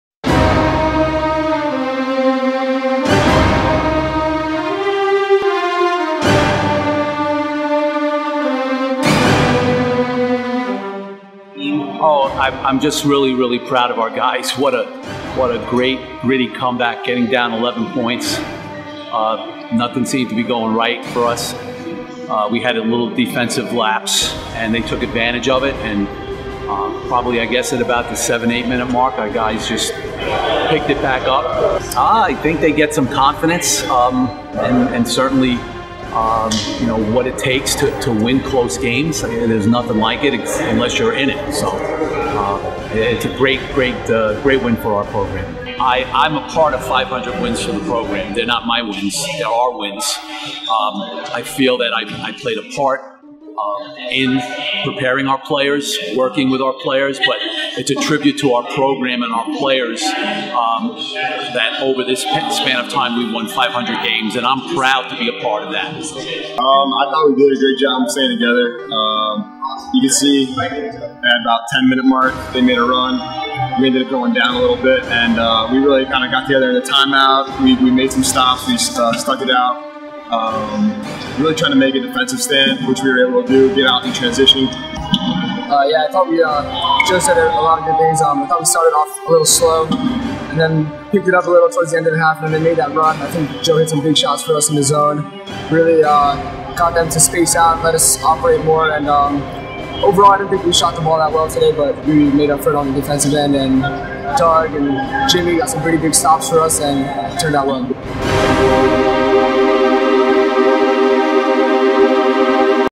Joe Nesci Gets Win 500 Against Brooklyn College 12-12
Co-Captains Ross Udine and Joe Timmes combined for 40 points as NYU battled back from a double-digit deficit in the 2nd Half to beat Brooklyn College 70-67. The victory was the 500th for Violet Head Coach Joe Nesci.